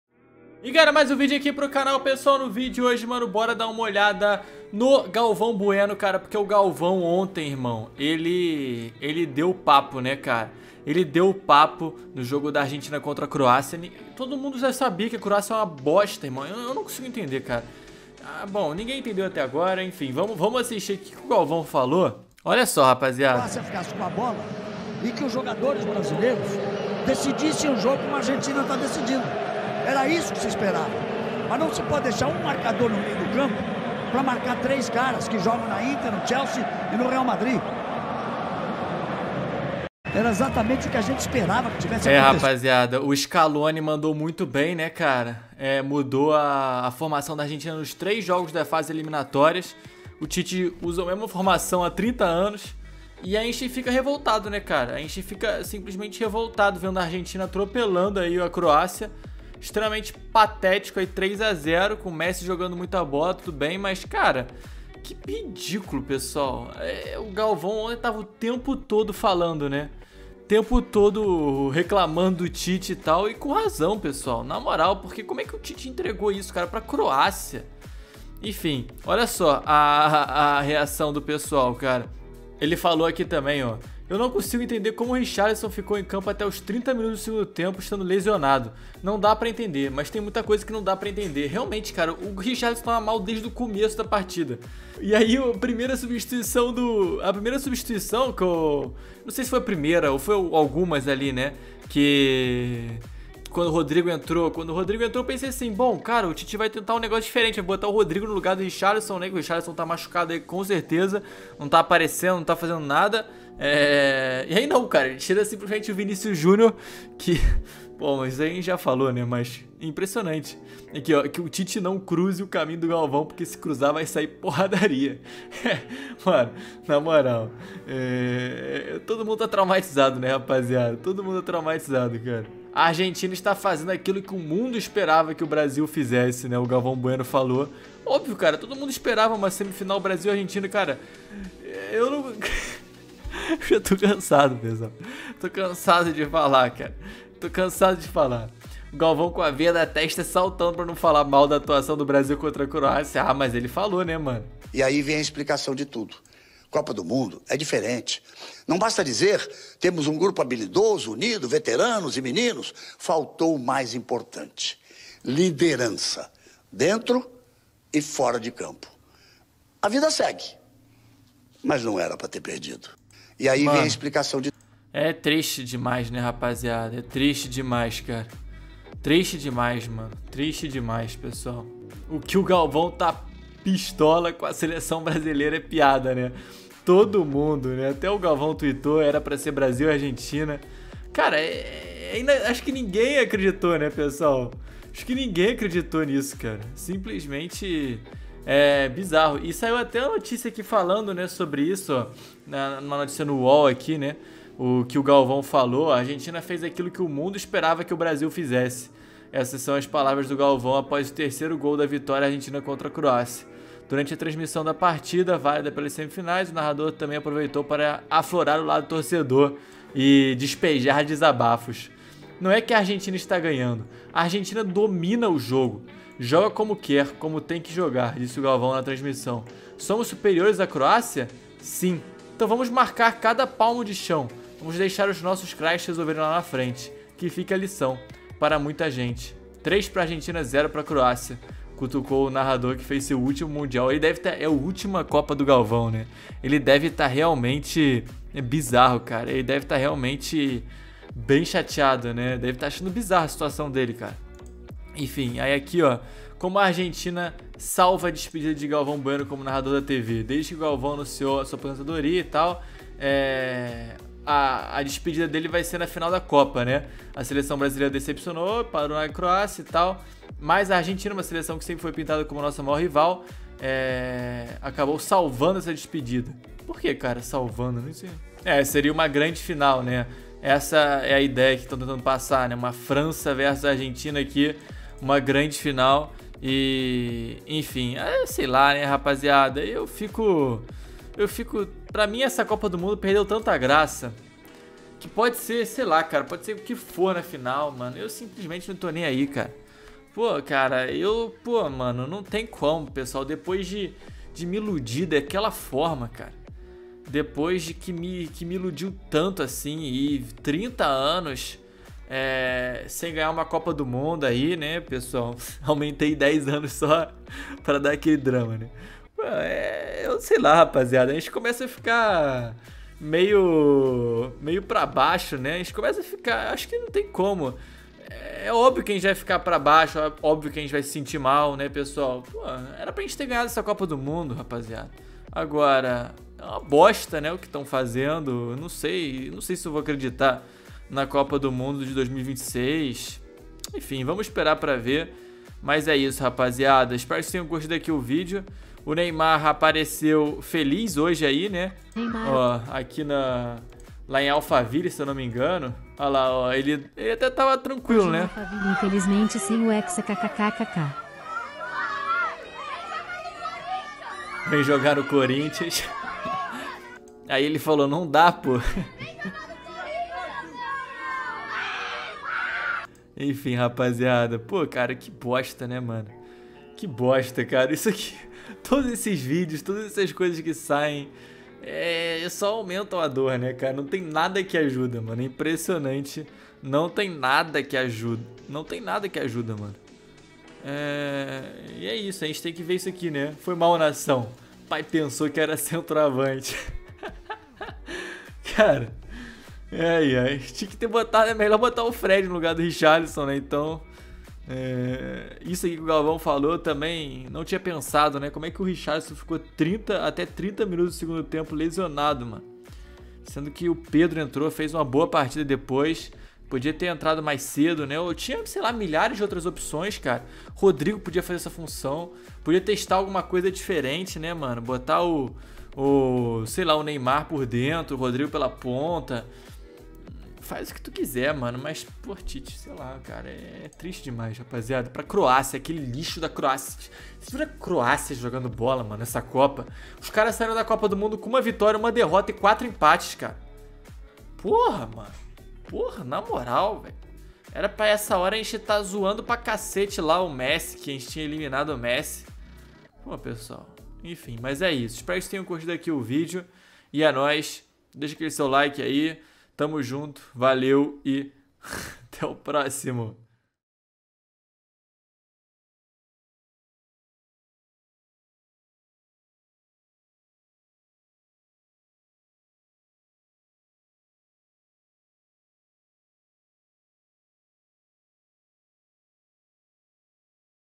GALVÃO REPRESENTOU TODOS OS BRASILEIROS!! GALVÃO PISTOLOU DEPOIS DA ARGENTINA ATROPELAR CROÁCIA!!
Vídeo sem intenção de violar os termos de direitos autorais, sendo assim, se sentir os direitos violados, nos envie um e-mail que o vídeo será retirado no mesmo instante!